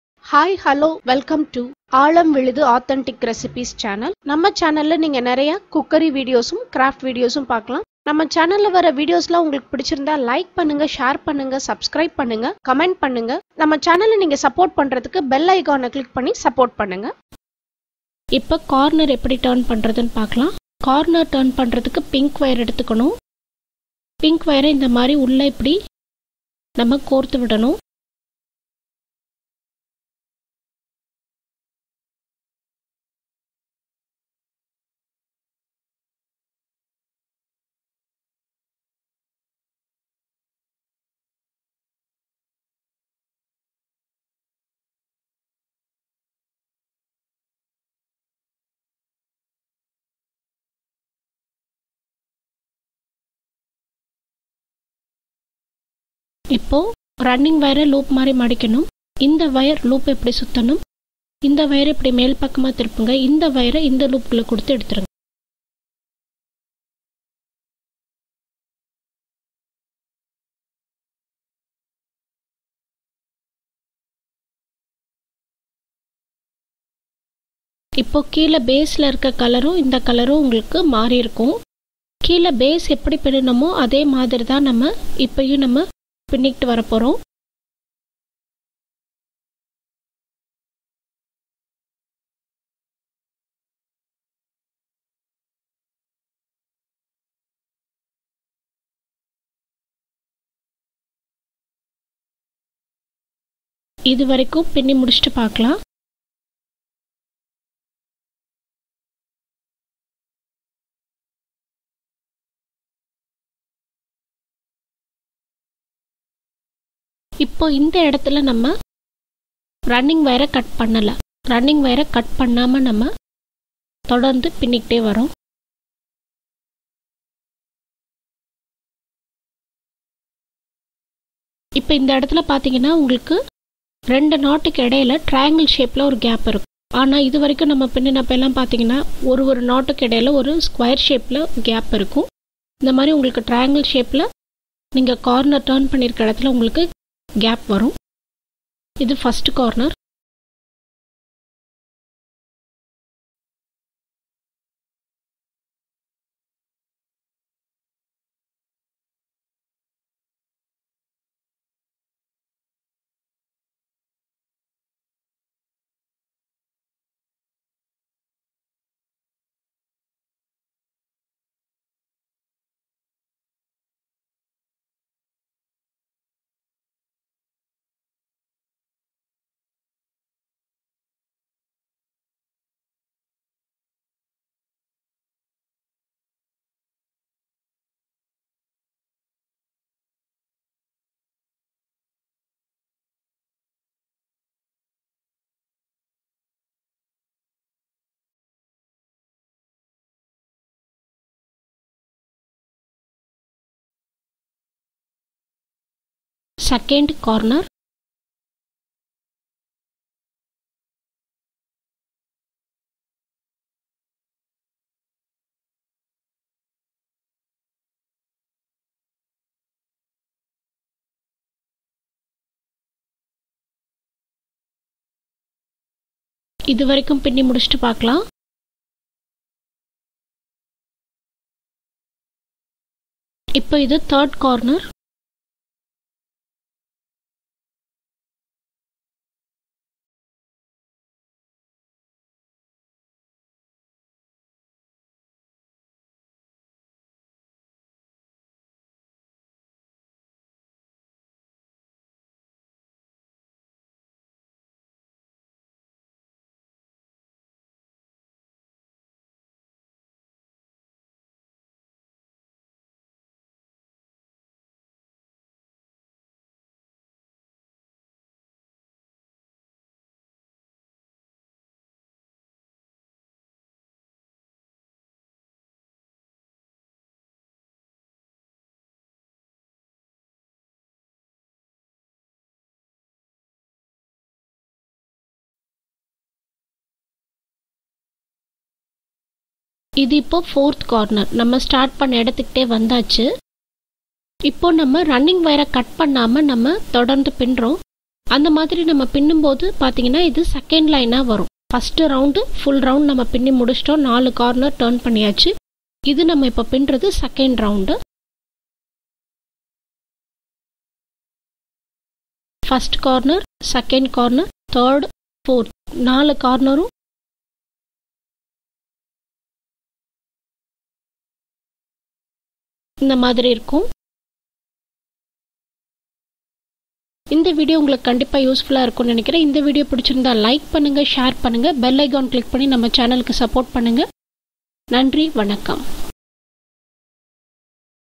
Wire Koodai CornerTurning Pinnal in tamil - Part 2
How To Make Easy Kattam Design Market Wire Basket[Bag] | ஒயர் கூடை சுலபமாக பின்னும் முறை  | wire koodai pinnuthal in tamil | wire koodai pinnuvathu eppadi